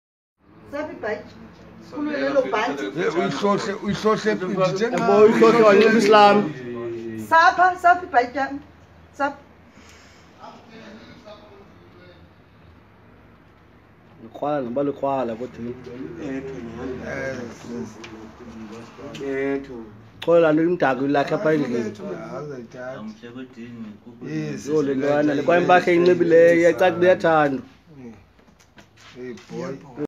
This young man stole tv plasma from her mother's house
Molwen bahlali lo ngu Sipho Tshazibane hlala zone 11 opposite bakery zone 5 likrwala elitsha bahlali bahlali June kodwa yinja lentwana imosha kokwayo ithengisa TV zasendlin nezinye izinto so plz be alert bayamosha abafana. Thanks again kwi Langa safety patrol nge visibility nange nxaso niyasebenza Madoda Enkosi